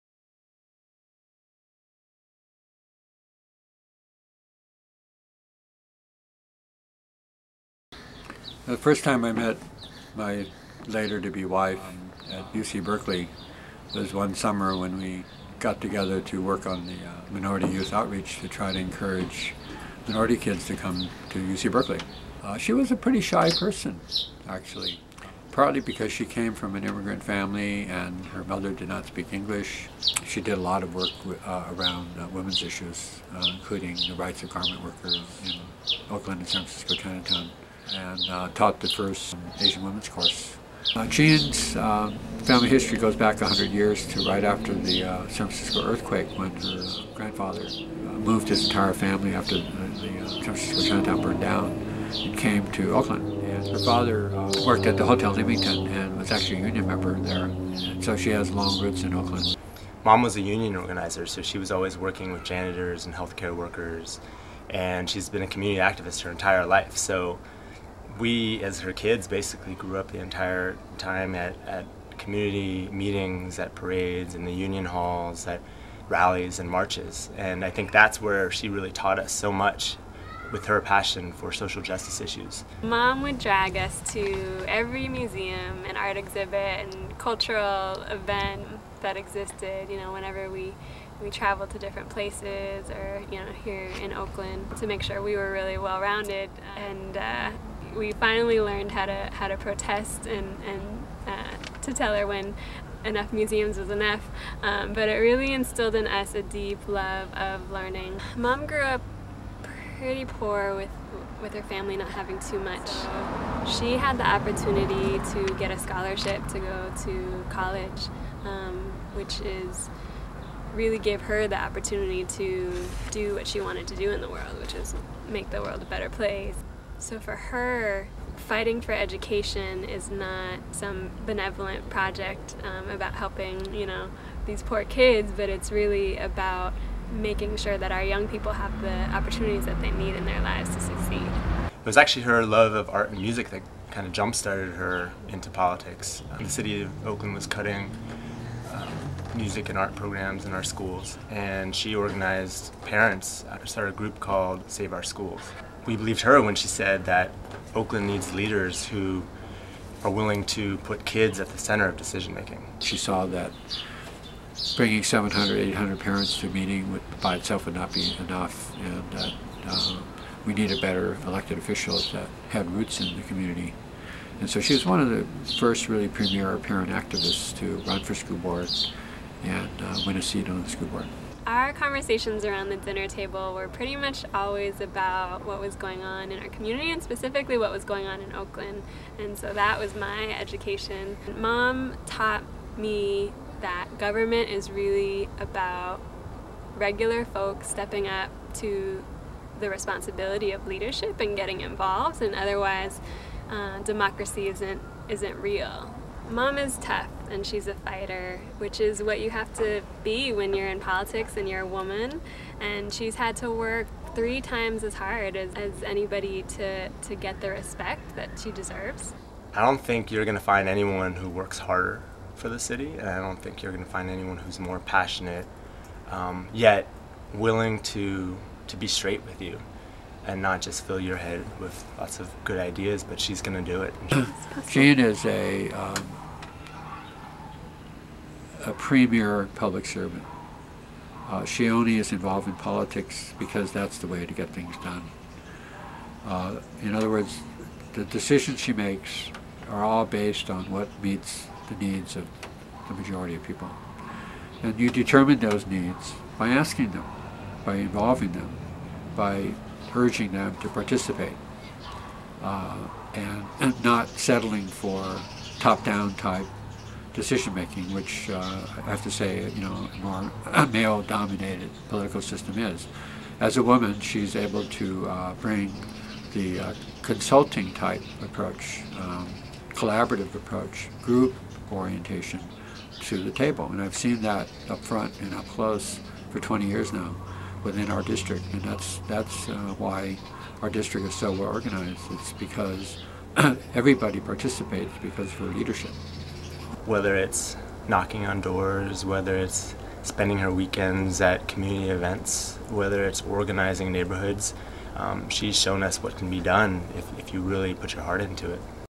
JEAN QUAN: Organizer, Mother, Educator & Public Servant
Jean's family -- Floyd, Will and Lailan -- share the real story of Oakland Councilmember Jean Quan and their family.

Jean Quan is running on November 2, 2010 to be Oakland's first woman Mayor and the country's first Asian American woman Mayor of a major U.S. city.